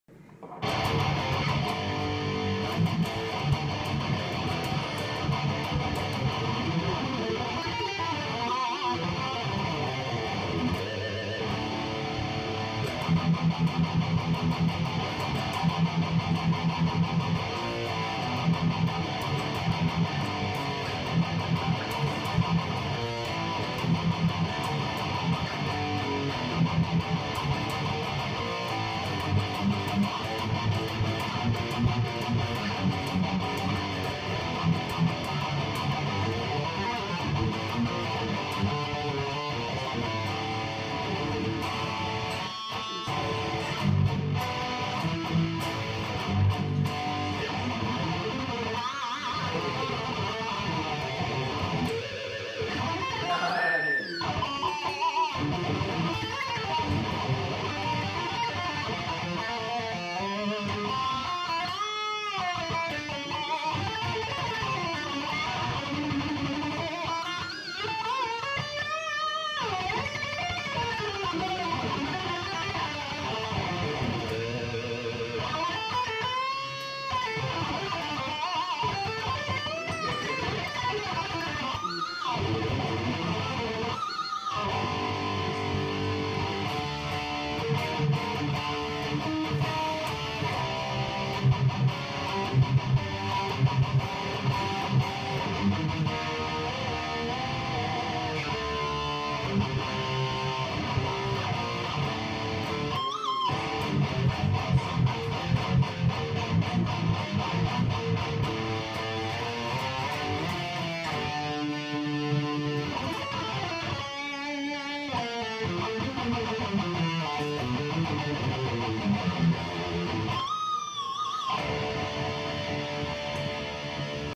Hd 500 poweramp and cab metal with Indiana ST Paul les Paul copy
Trying out a power amp and cab with my pod hd500